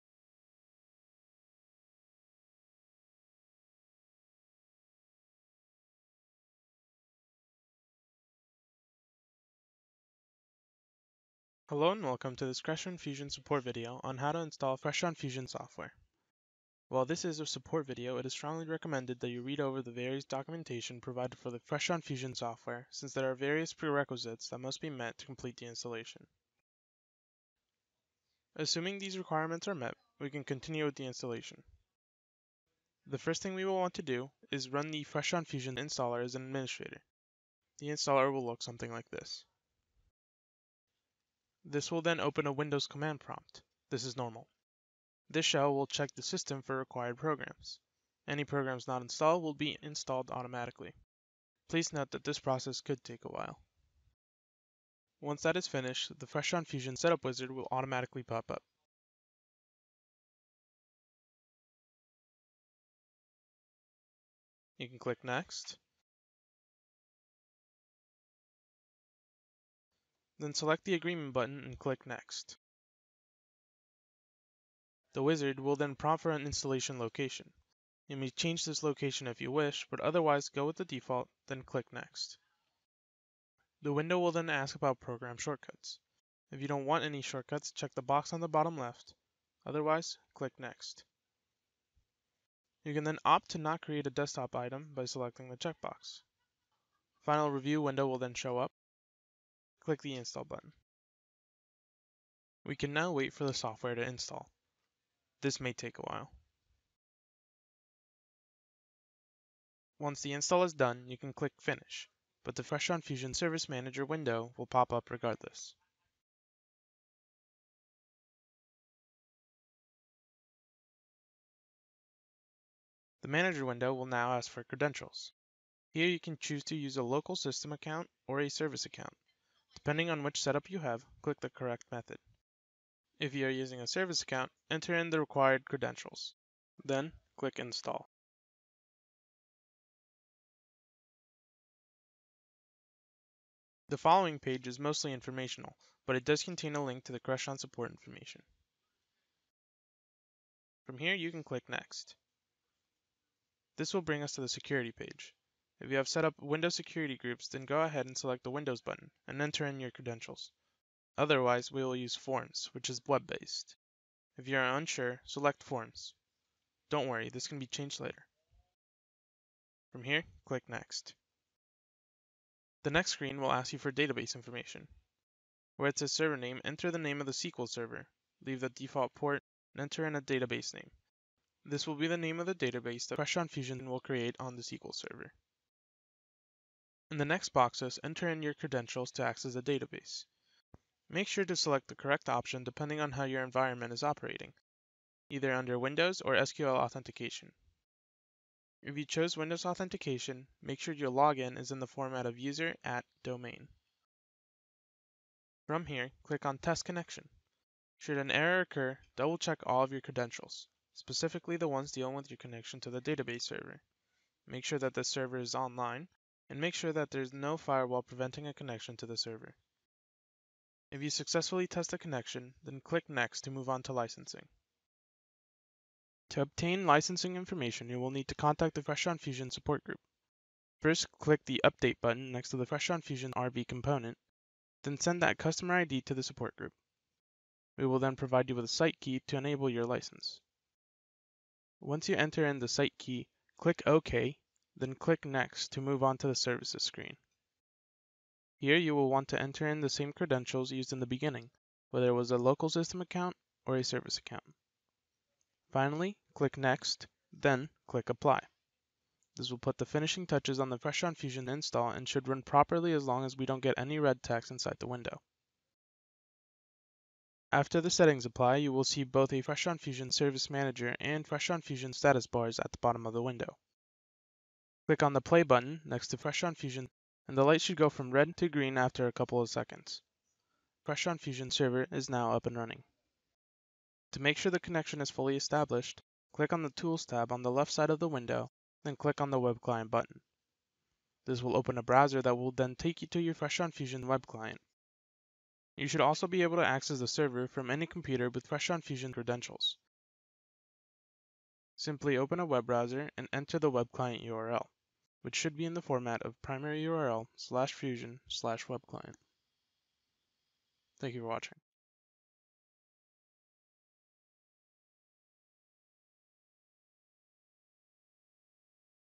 Crestron Fusion® Software: Install Guide
This video is designed to guide you through the process of installing Crestron Fusion® through a local system and forms authentication.